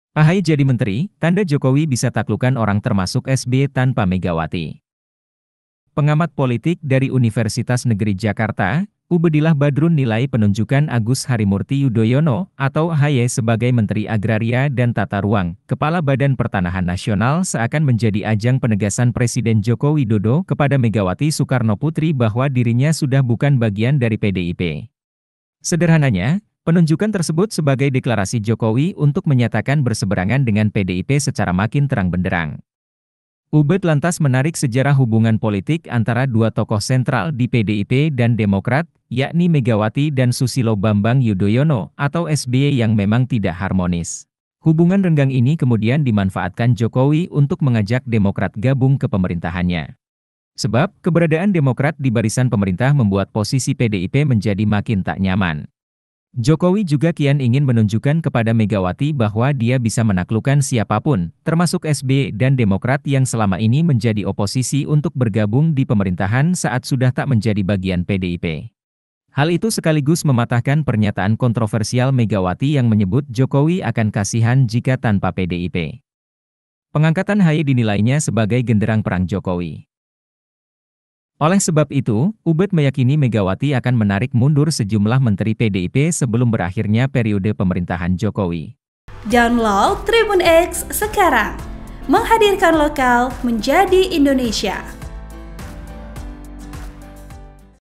Jokowi Patahkan Omongan Megawati : Akan Kasihan Jika Tanpa PDIP, Jokowi Bisa Tarik SBY Tampa Mega
Pengamat politik dari Universitas Negeri Jakarta, Ubedilah Badrun nilai penunjukan Agus Harimurti Yudhoyono atau AHY sebagai Menteri Agraria dan Tata Ruang/Kepala Badan Pertanahan Nasional seakan menjadi ajang penegasan Presiden Joko Widodo kepada Megawati Soekarnoputri bahwa dirinya sudah bukan bagian dari PDIP.

(dic)